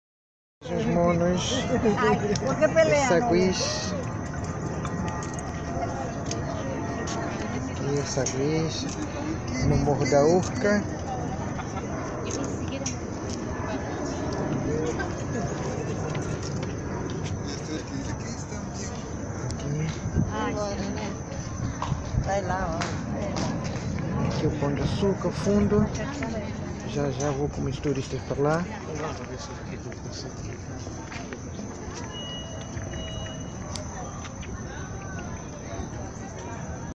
Os saguis no Complexo do Pão de Açúcar / The marmosets on Sugar Loaf Complex
O Pão de Açúcar é um complexo de morros localizado no bairro da Urca na cidade do Rio de Janeiro, é composto pelo  Morro do Pão de Açúcar, Morro da Urca e pelo Morro da Babilônia.